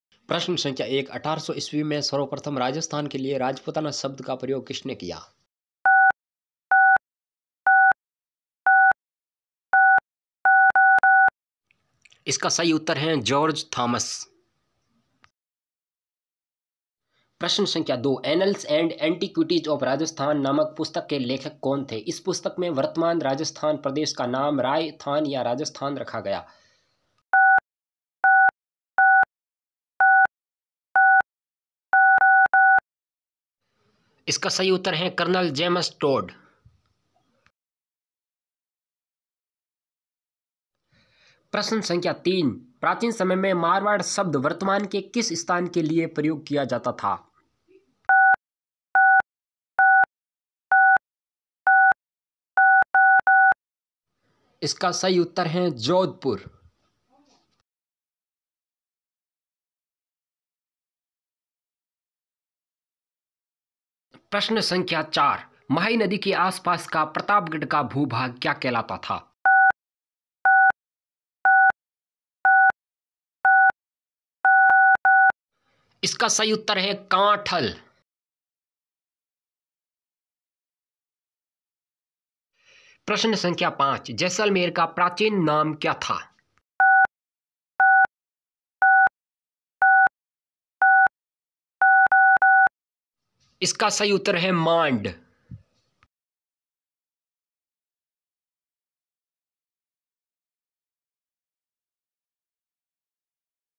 राजस्थान सामान्य ज्ञान प्रश्नोत्तरी। rajasthangk
इस वीडियो में राजस्थान सामान्य ज्ञान के पांच प्रश्नोतर है ।यह वीडियो राजस्थान सामान्य ज्ञान अध्ययन में सहायक सिद्ध होगा।